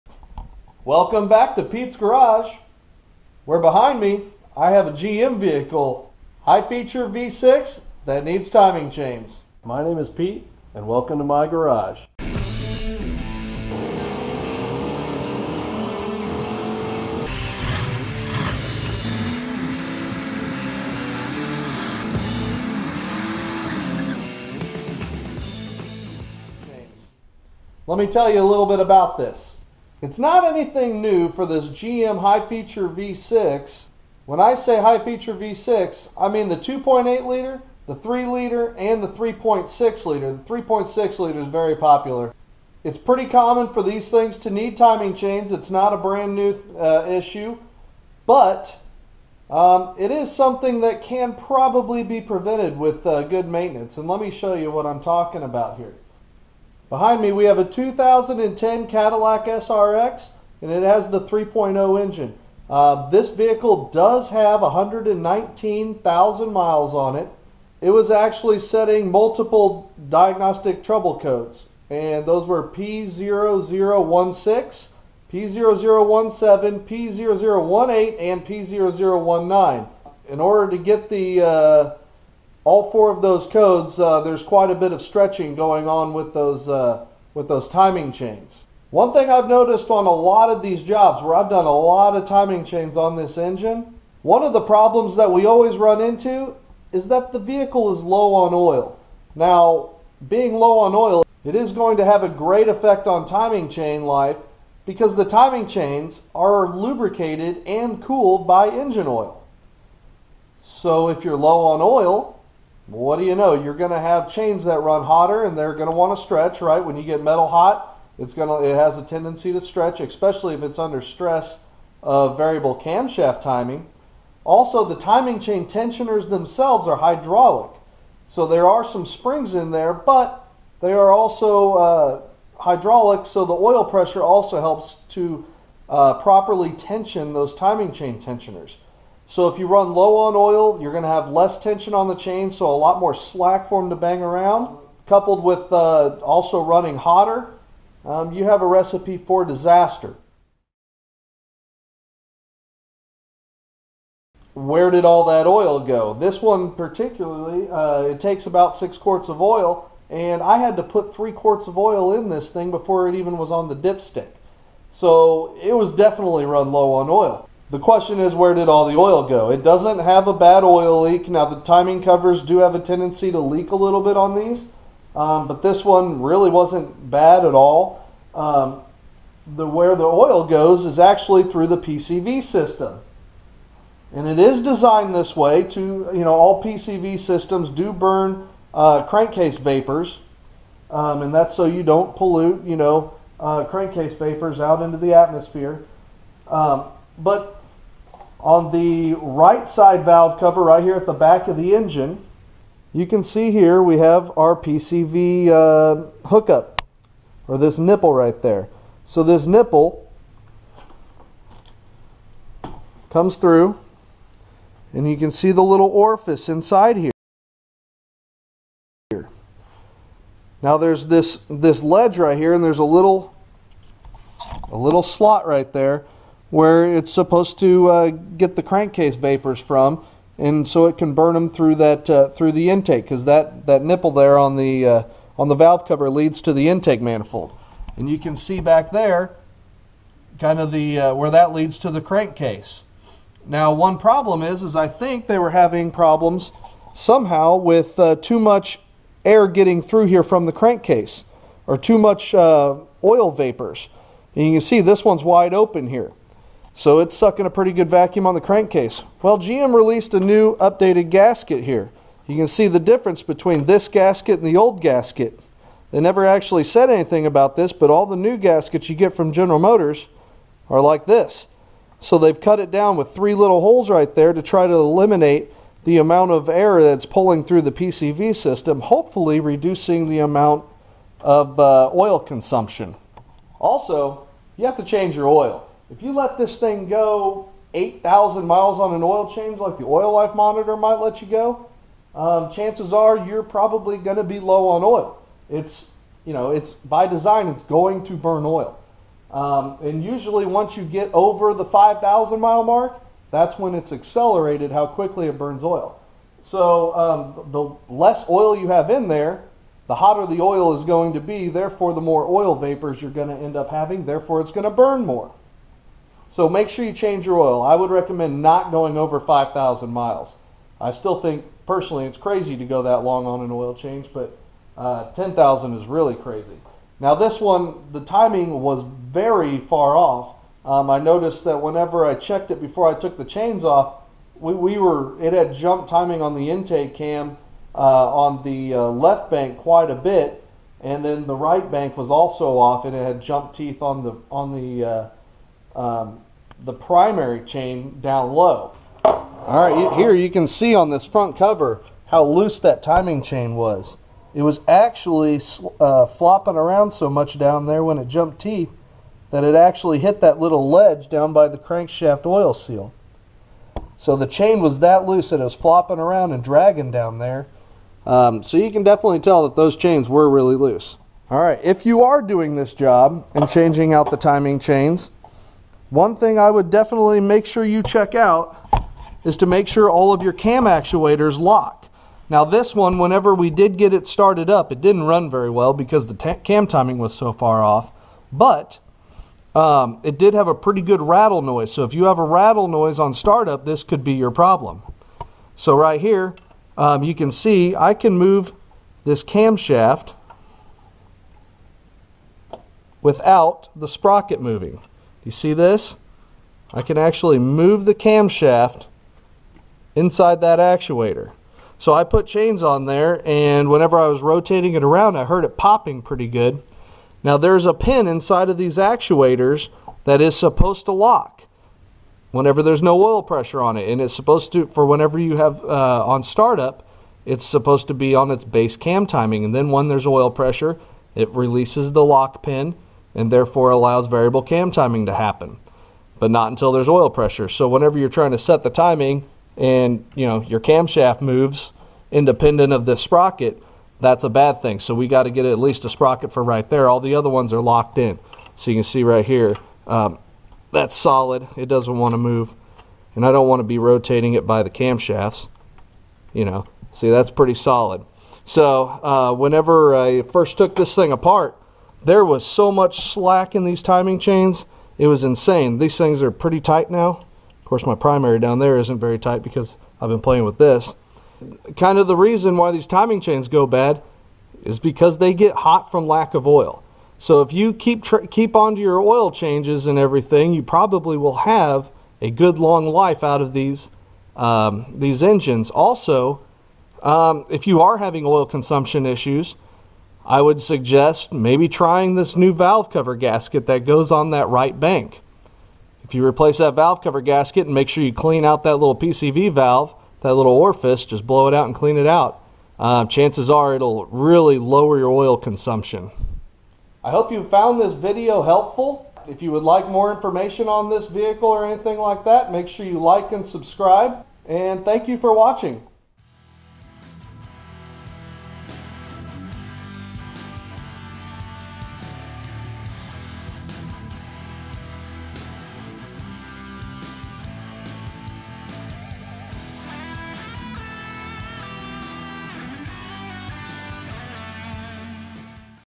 Why GM 3.0 and 3.6 V6 Timing Chains Fail
In this video I go over why the GM High Feature V6 timing chains have failures. That's right, they're not just junk. There's a reason they fail!

GM Timing chain kit with updated valve cover gasket
GM Silicone RTV
Camshaft holding tools